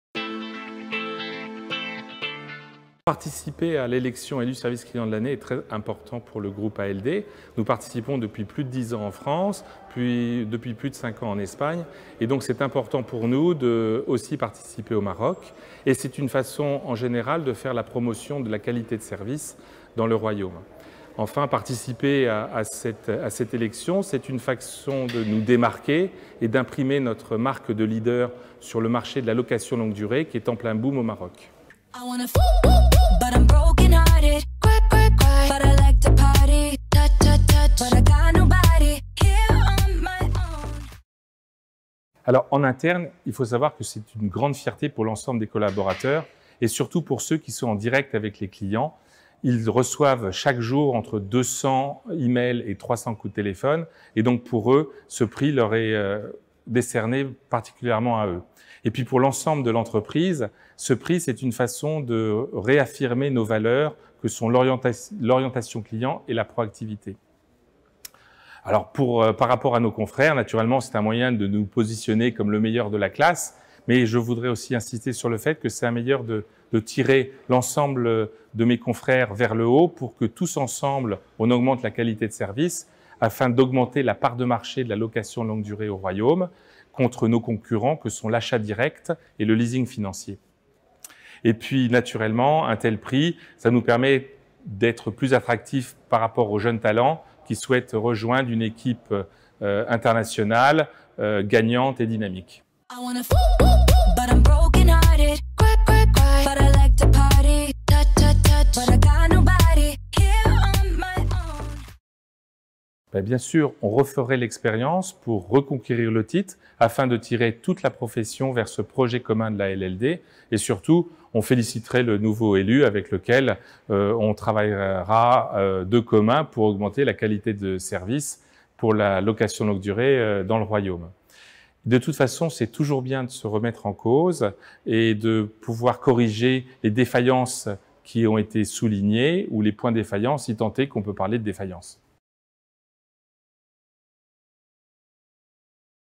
Interview de Monsieur Philippe VALIGNY, directeur général d'ALD automotive Maroc
Retour d'expérience sur différentes participations à l’Élection du Service Client de l'Année Maroc dans un secteur en plein essor, la location de voitures longue durée